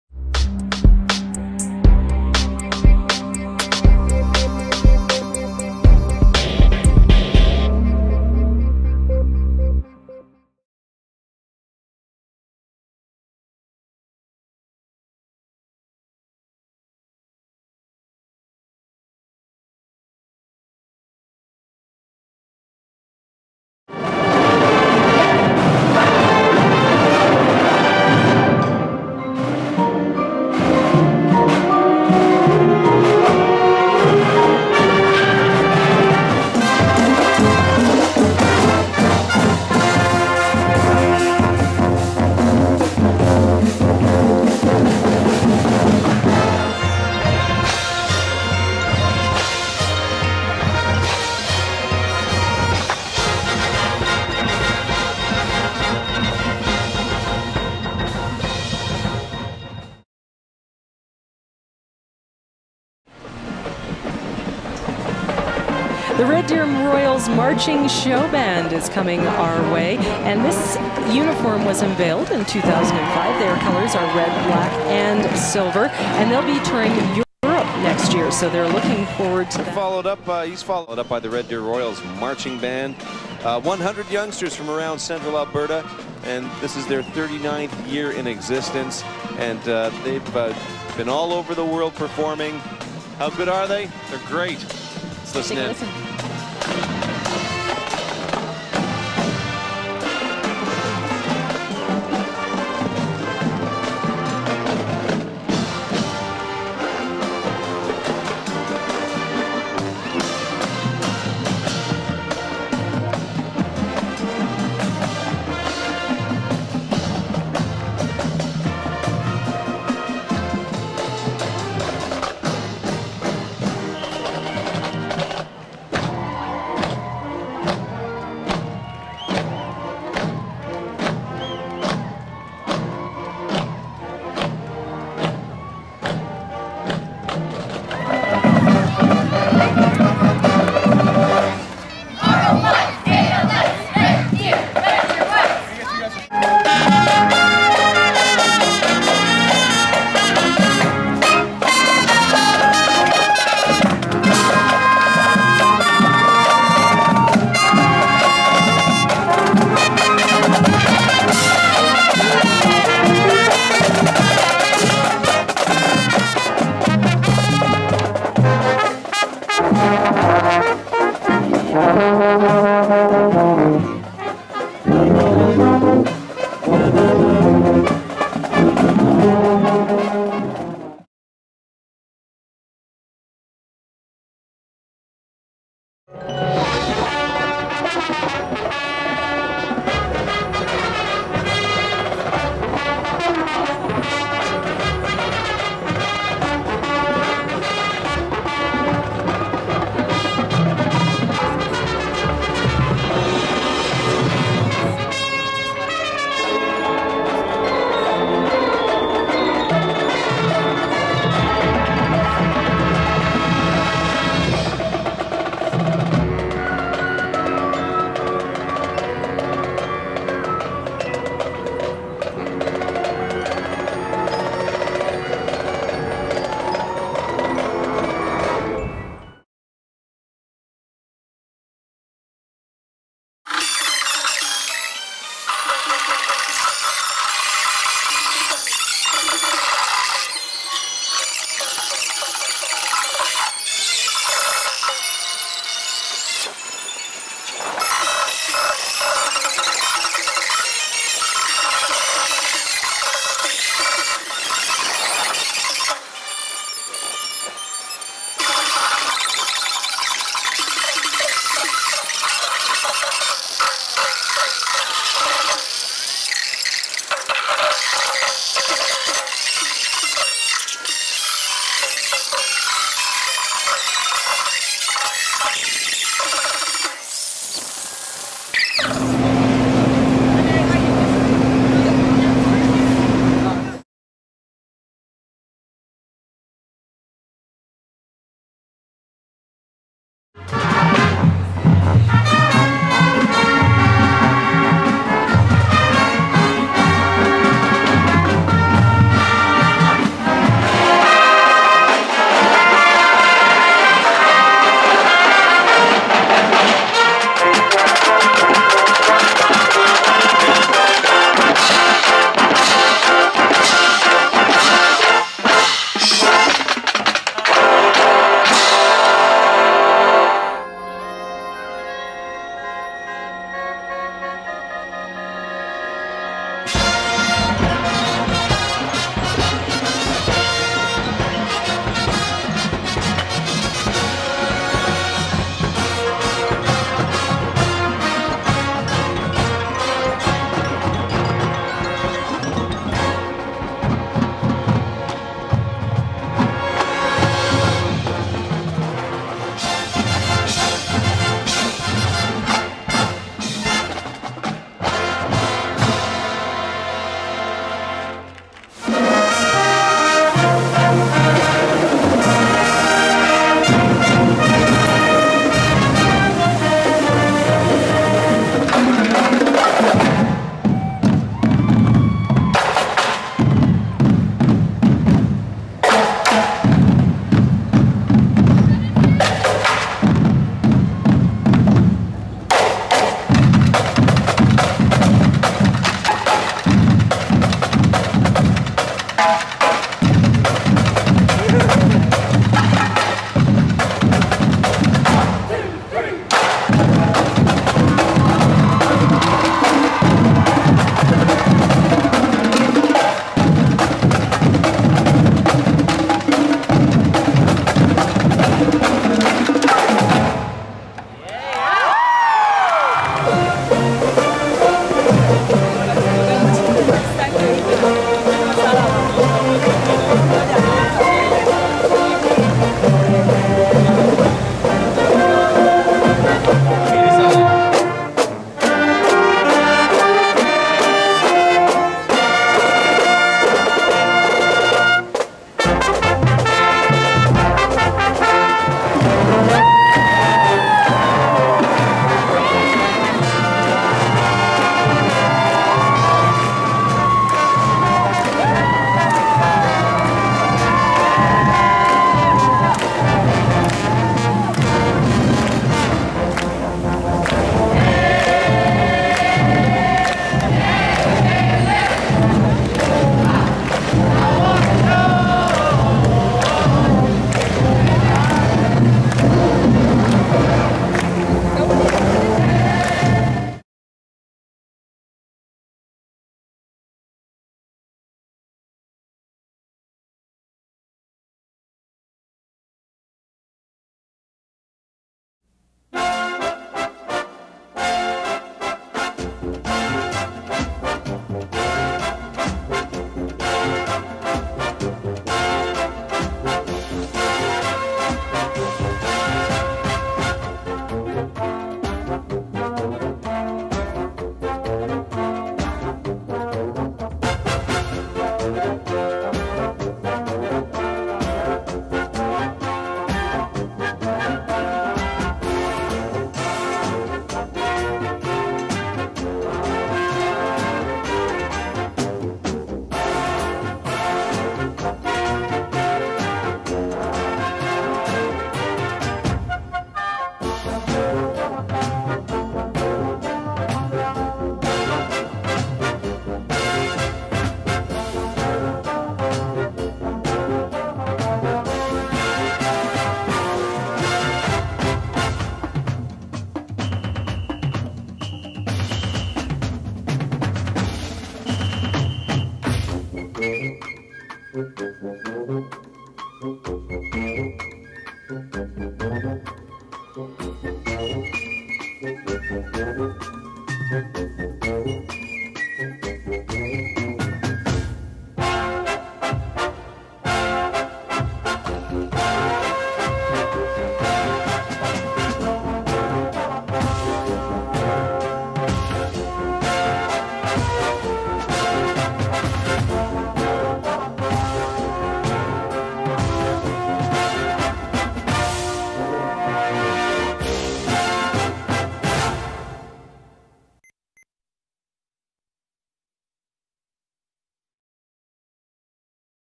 Red Deer Royals - 2012 Recruitment Video - in HD
The RED DEER ROYALS Concert Band and Marching Show Band is now welcoming new members for the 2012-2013 year!
for more information.

The Royals are 100 young people aged 12 to 20 from across Central Alberta.
Known for variety of repertoire and originality of presentation, the Royals lay it down in the concert hall, on the field, on the floor and in the street.